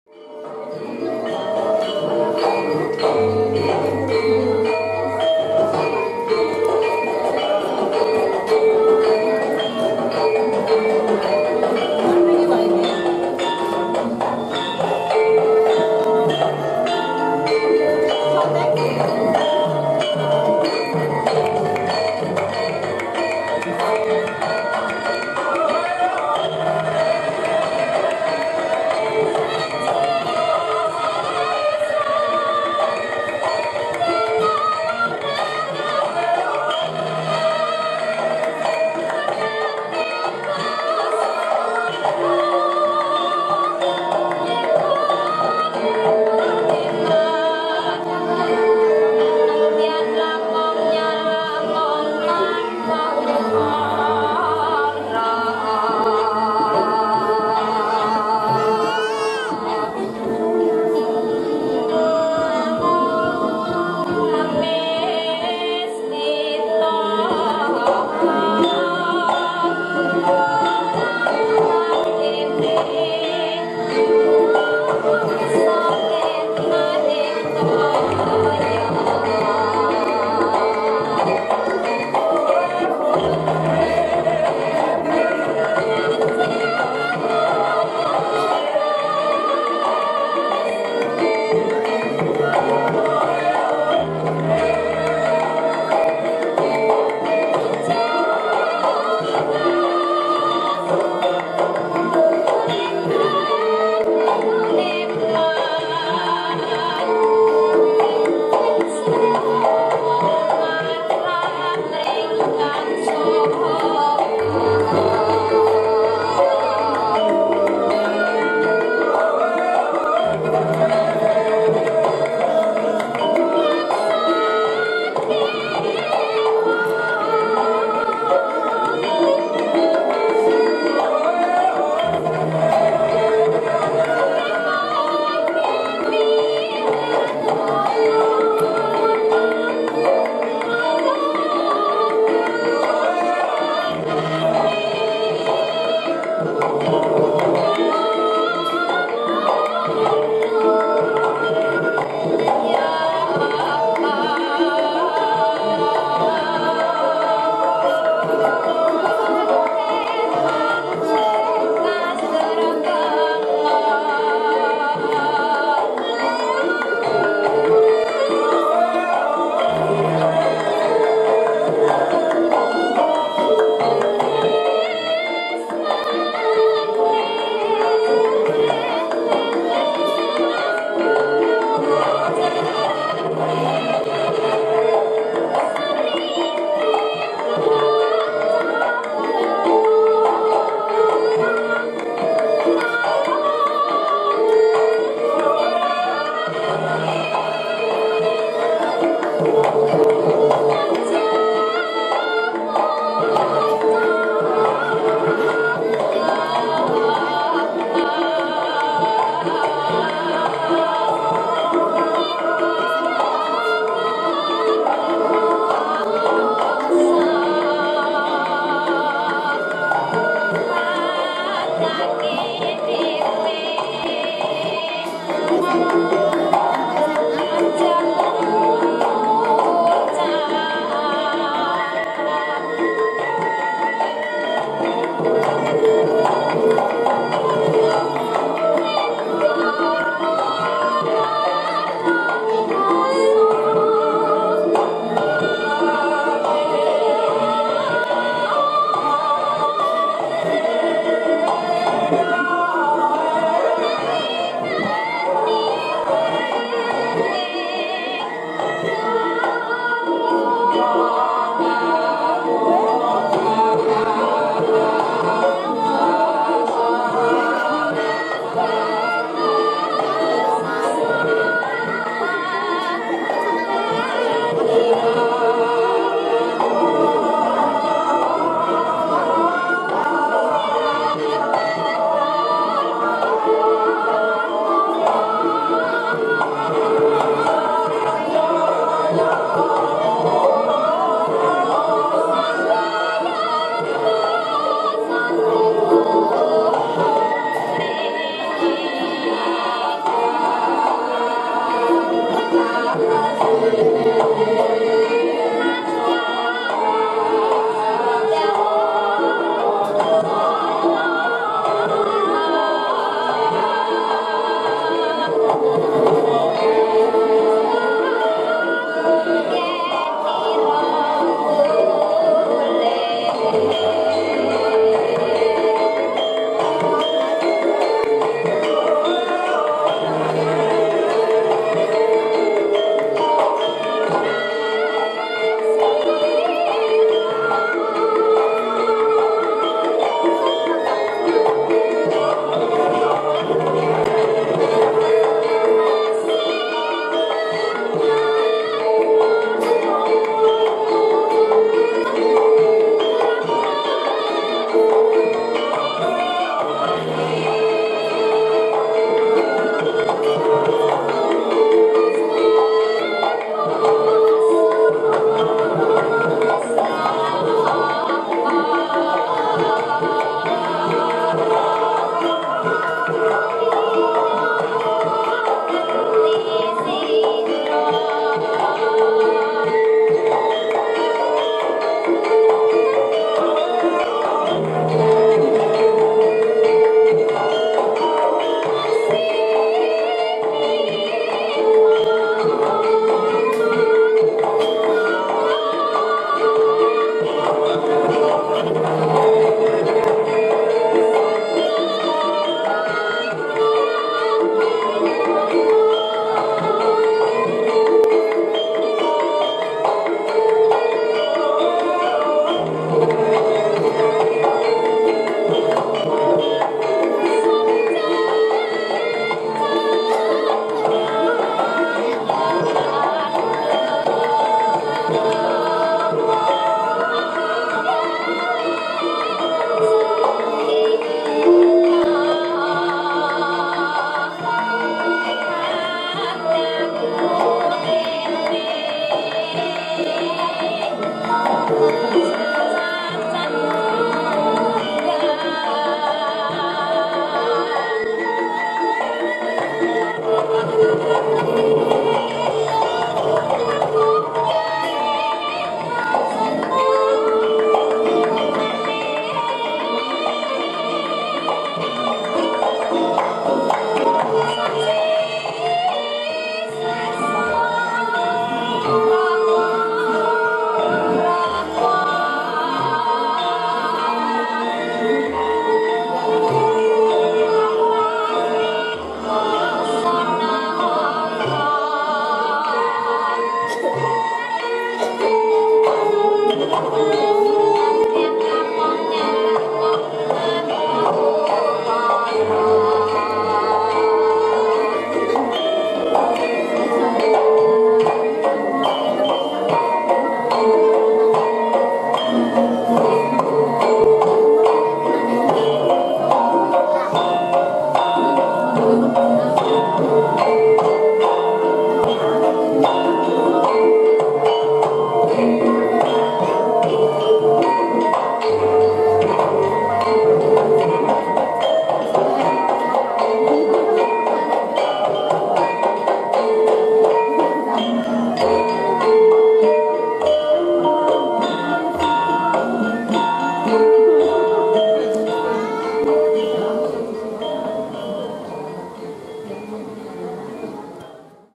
Tari Gambyong Besani diacara Resepsi Pernikahan
Pentas tari Gambyong Menghibur Pengantin